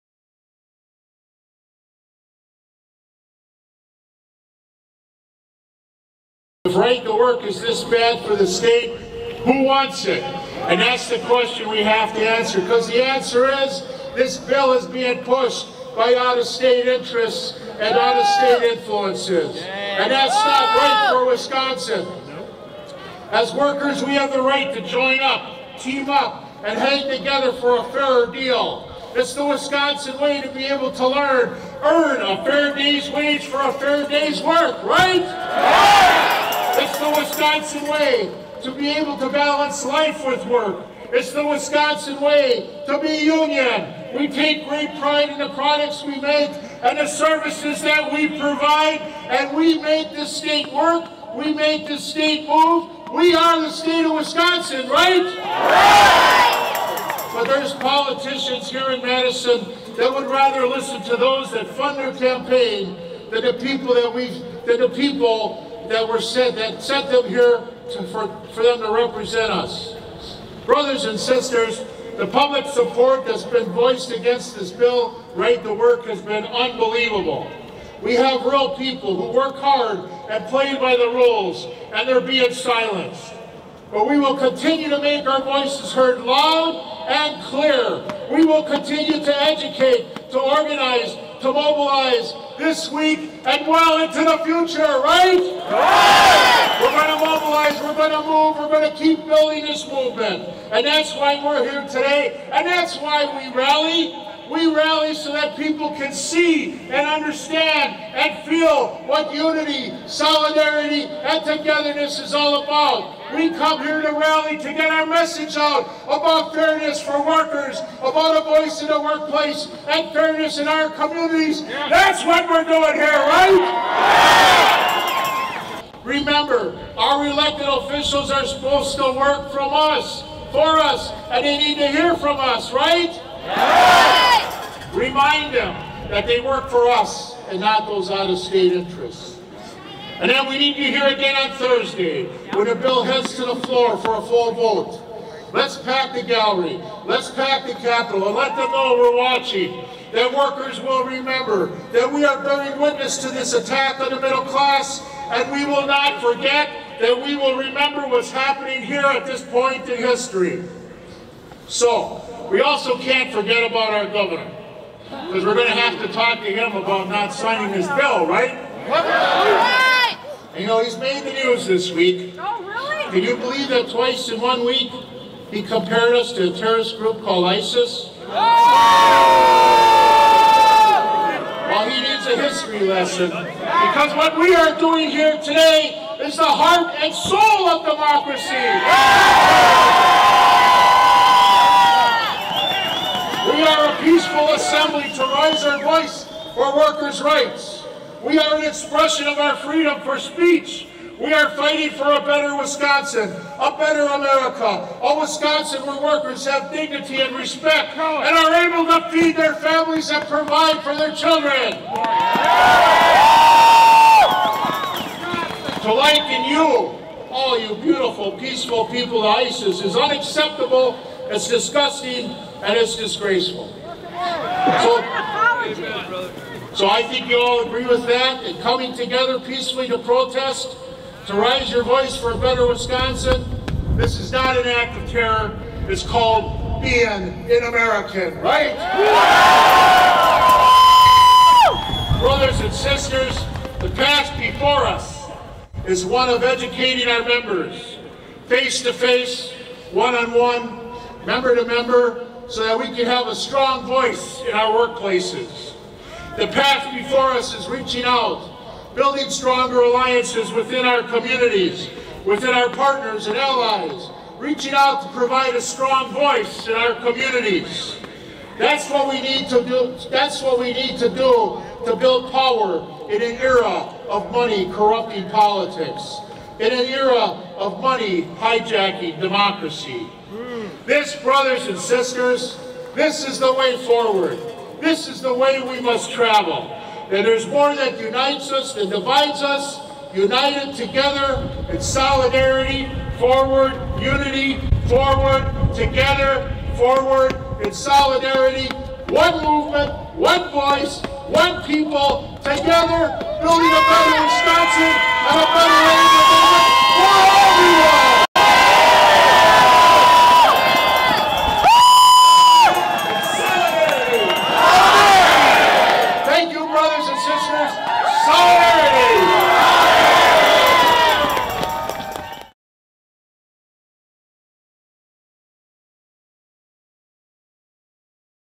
Phil Neuenfeldt President WI AFL CIO
Phil Neuenfeldt, speaking at Right To Work protest in Madison, WI on  Feb, 28,2015.